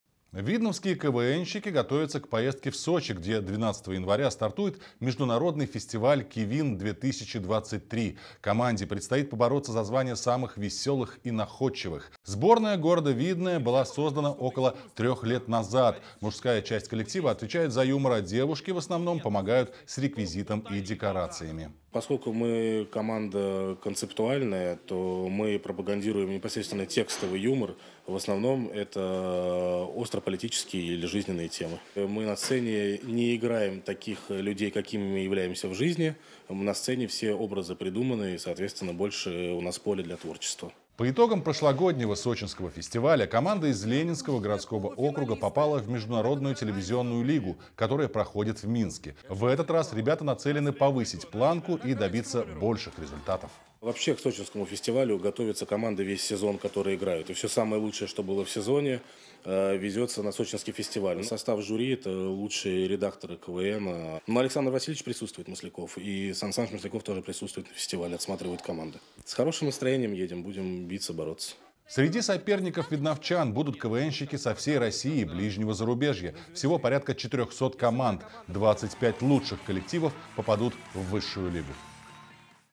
НАВСТРЕЧУ КИВИНУ
Видновские квнщики готовятся к поездке в Сочи, где 12-го января стартует международный фестиваль «Кивин» - 2023. Команде предстоит побороться за звание самых весёлых и находчивых.